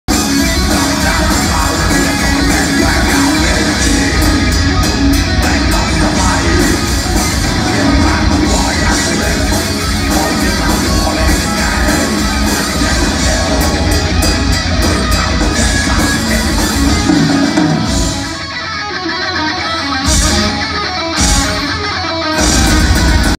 Kalmah mosh pit in New York
Here’s a crazy mosh pit!  Only in New York!